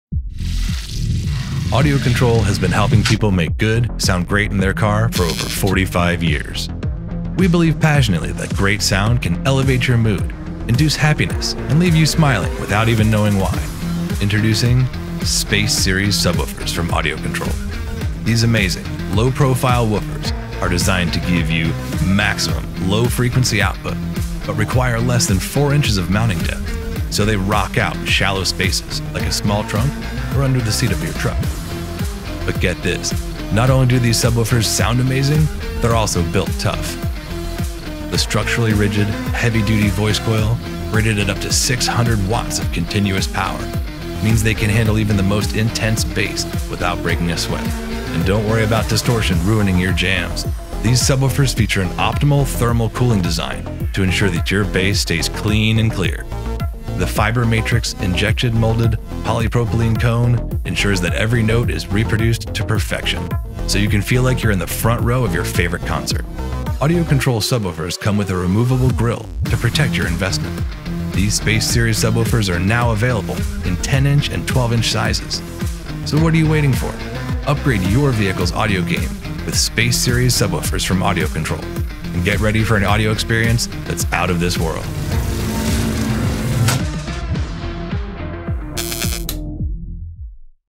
Audio Control Space Series Low-Profile Subwoofers
Introducing AudioControl's 400 Watt Space Series Low-Profile Subwoofers, the perfect addition to your car audio system. With a power output of 400 watts, these subwoofers will provide you with the rich, deep bass you've been searching for. And with your choice of 2-OHM or 4-OHM impedance, they're incredibly versatile and can be customized to fit your specific needs.

Available in both 10" and 12" sizes, these low-profile subwoofers are designed to fit into even the tightest spaces, without sacrificing sound quality. And with their sleek, modern design, they'll look great in any vehicle.

So why wait? Upgrade your car audio system today with AudioControl's 400 Watt Space Series Low-Profile Subwoofers and experience the difference for yourself!

10" 400 Watt Space Series Low-Profile Subwoofer | 2-OHM & 4-OHM

12" 400 Watt Space Series Low-Profile Subwoofer | 2-OHM & 4-OHM